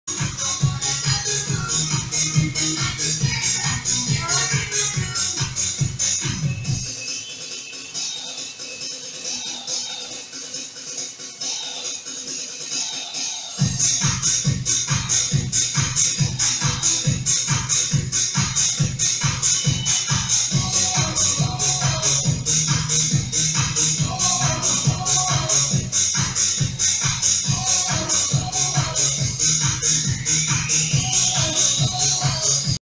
Aerobics_वीर सरोहा अपने दोस्त की अकैडमी में एरोबिक्स कराते हुए कुछ देर के लिए।
वीर सरोहा अपने दोस्त की अकैडमी में एरोबिक्स कराते हुए कुछ देर के लिए।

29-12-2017